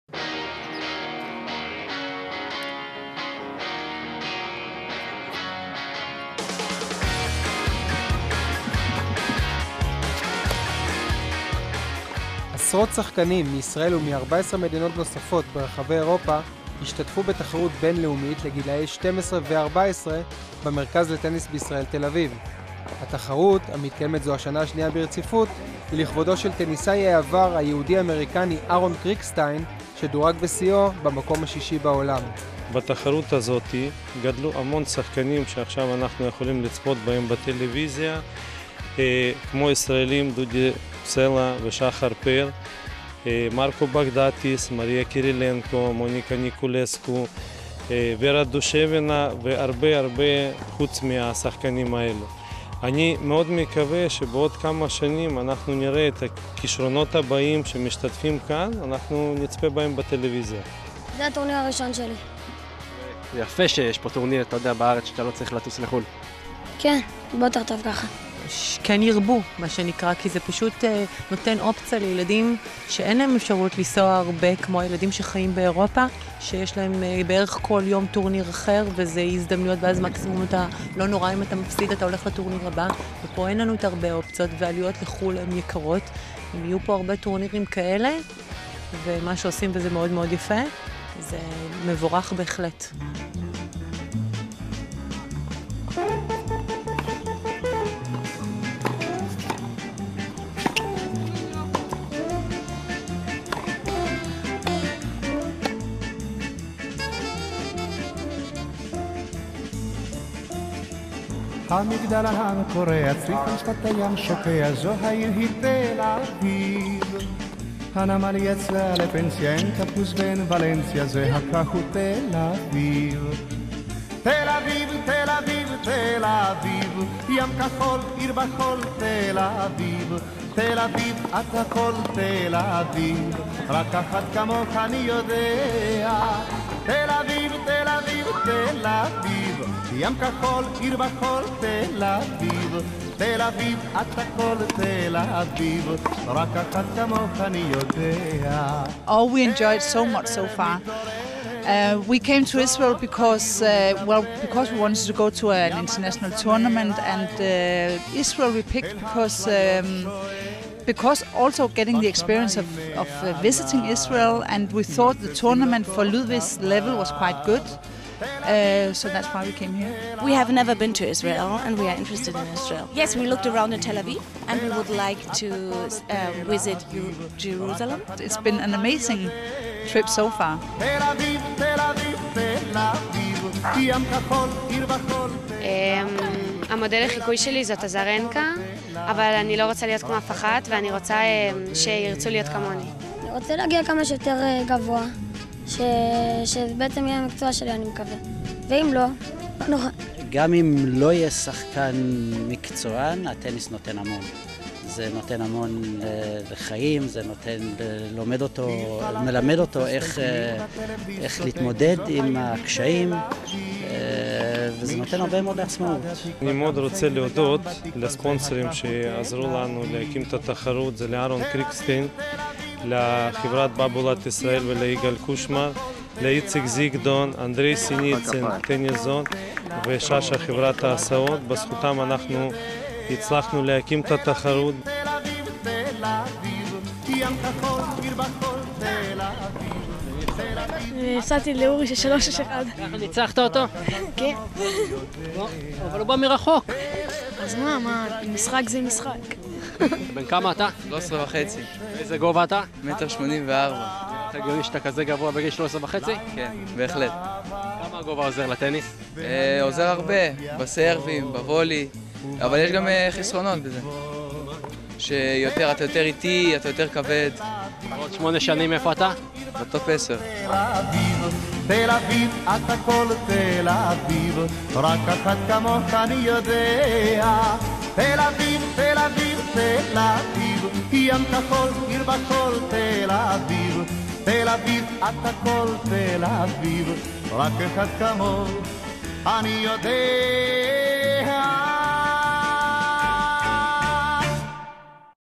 טורניר טניס בינלאומי לכבוד אהרון קריקסטיין
תחרות אירופאית לגילאי 14, שהתקיימה במארס 2012 במרכז לטניס בישראל -- תל אביב, לכבוד טניסאי העבר היהודי-אמריקני שדורג בשיאו במקום השישי בעולם